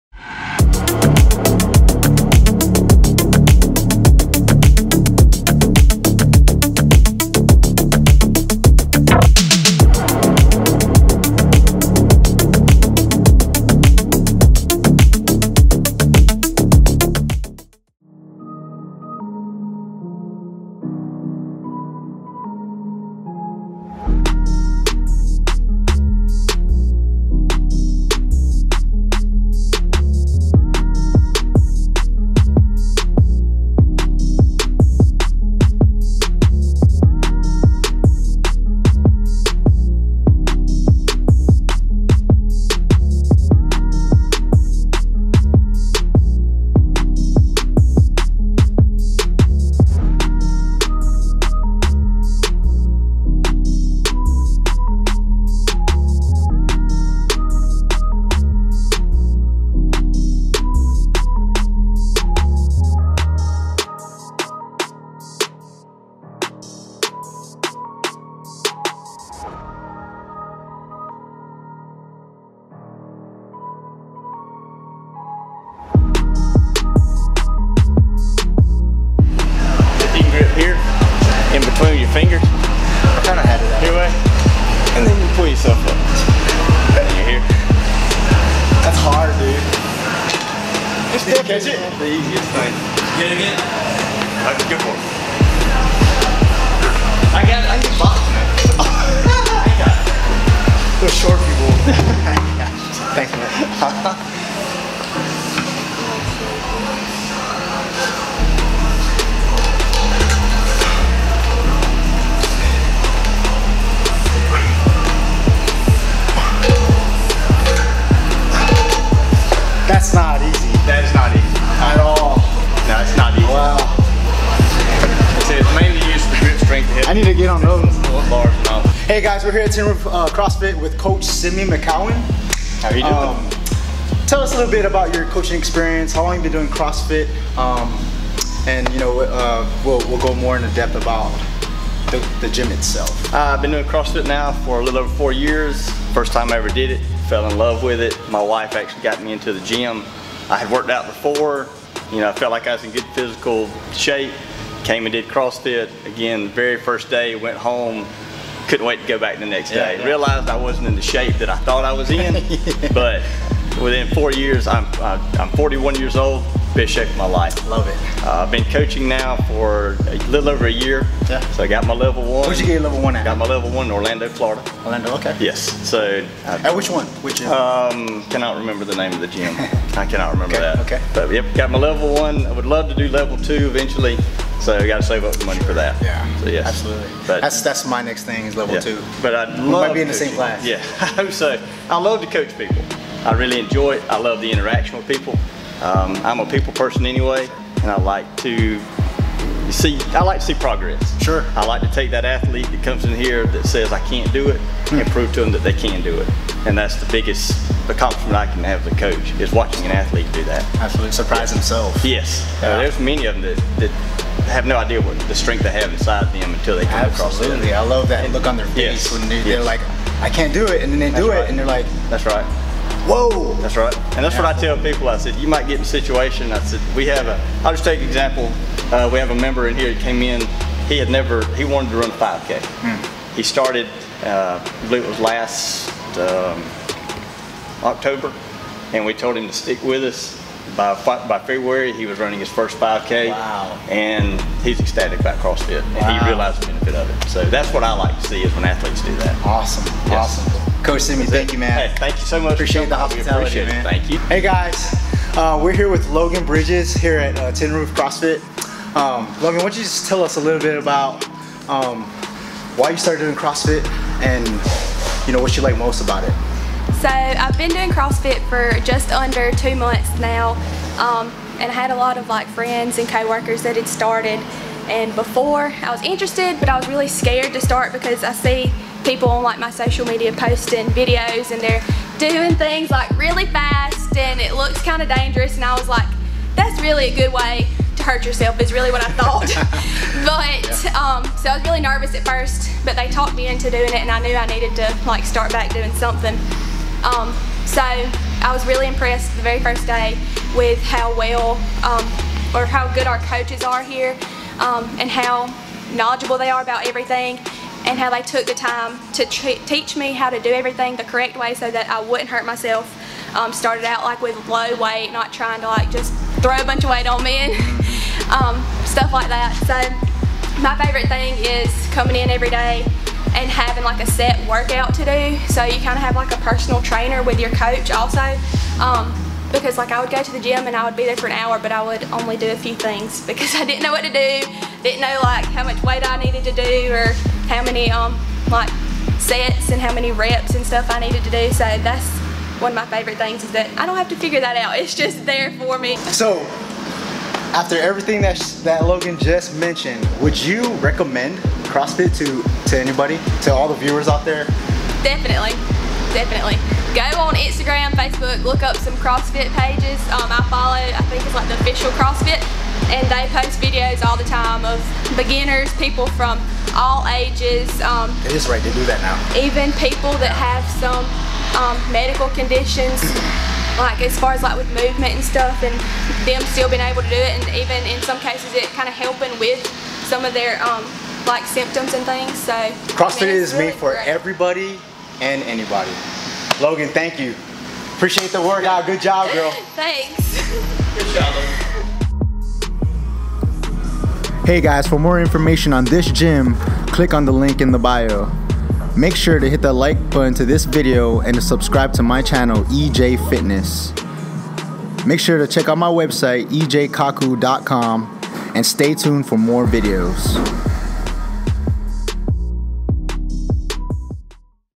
Tin Roof Crossfit Vlog #3
In Vlog #3 we attend a class at Tin Roof Crossfit in Tifton, Ga. For more information about their gym and services, please go For more on EJ Kaku Fitness go to my Stay tuned for more videos where we travel to other crossfit gyms across the South East.